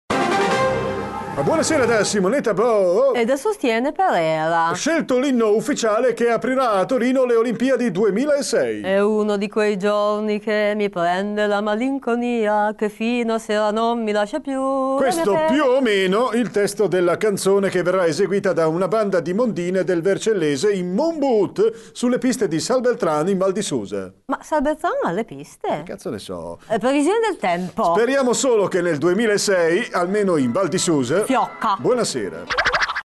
363 TG FINE L'anno x le olimpiadi - Ciau Bale -
TG THIE'
la parodia del TG regionale del Piemonte. con Simonetta Boh e Sostiene Perera